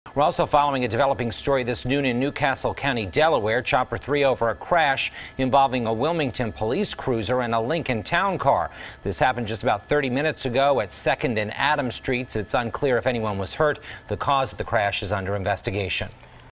Police Cruiser Involved In Crash In Wilmington
Crews were dispatched to 2nd Street and Adams Street.